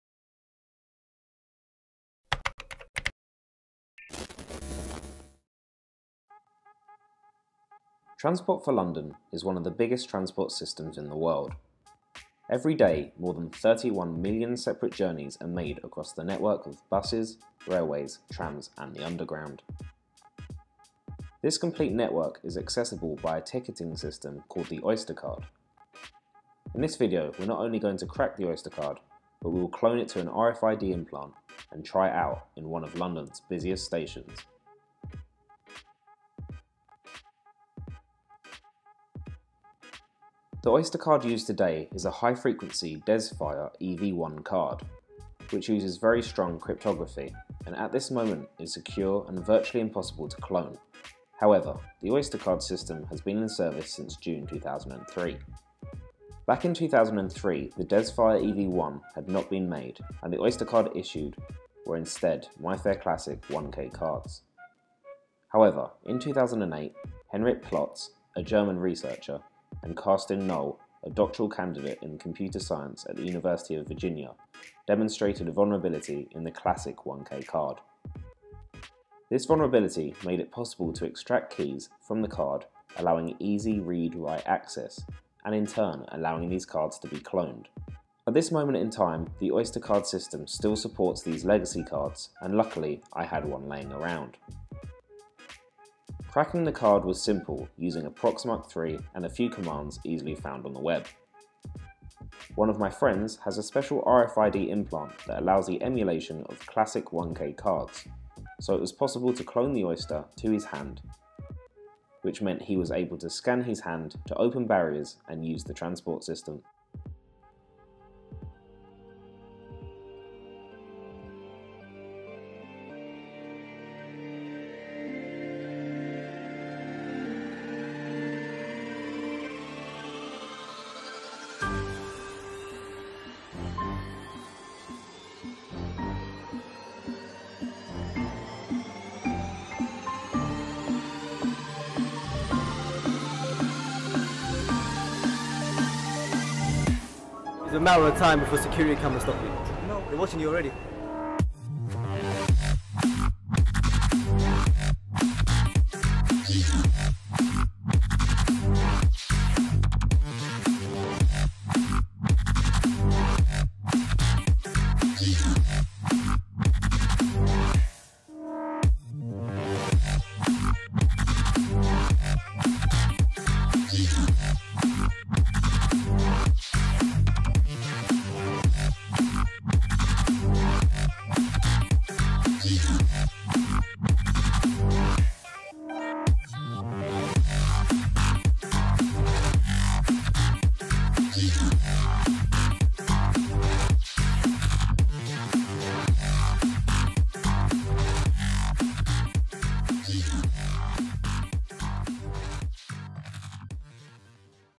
Oyster Hack
Hacking and Cloning London oyster card to an RFID implant.